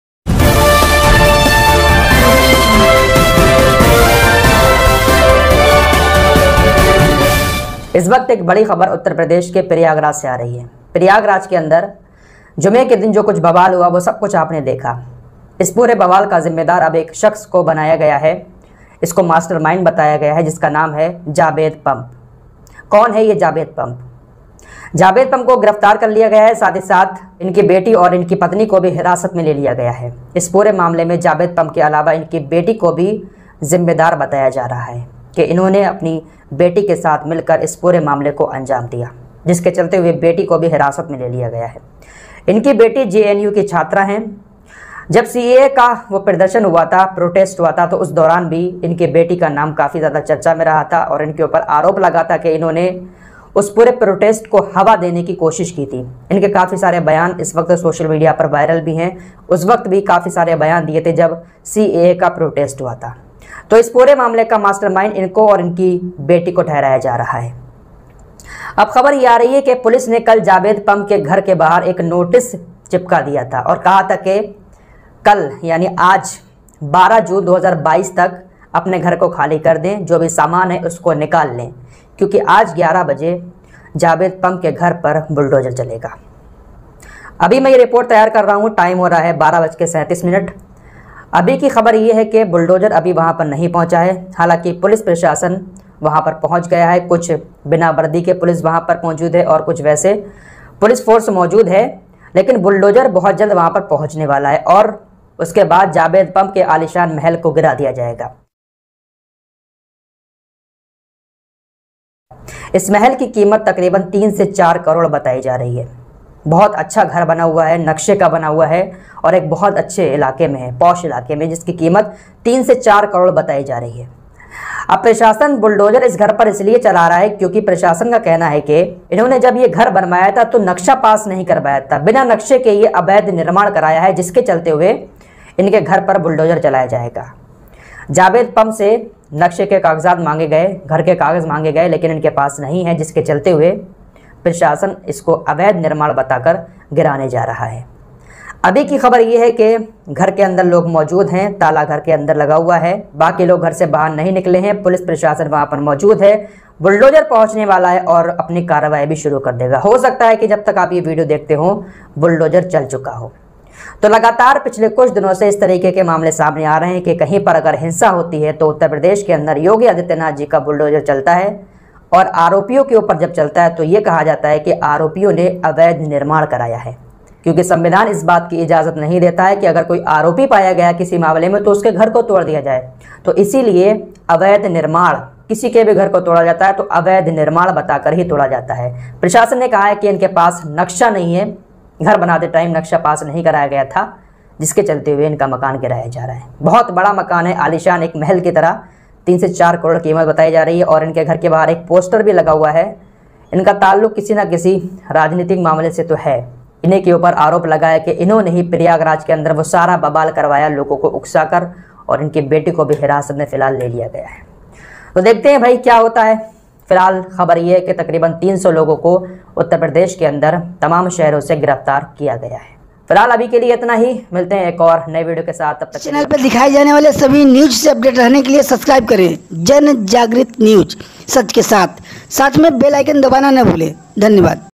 प्रयागराज में पकड़े गए आरोपी का, करोड़ों का घर बुलडोजर के द्वारा तोड़े जाने के लिए,पहुंची प्रशासन !
प्रयागराज में जुम्मे के दिन हुए मामले में पकड़े गए आरोपी का, करोड़ों का घर बुलडोजर के द्वारा तोड़े जाने के लिए,पहुंचे प्रशासन व,कर्मचारी।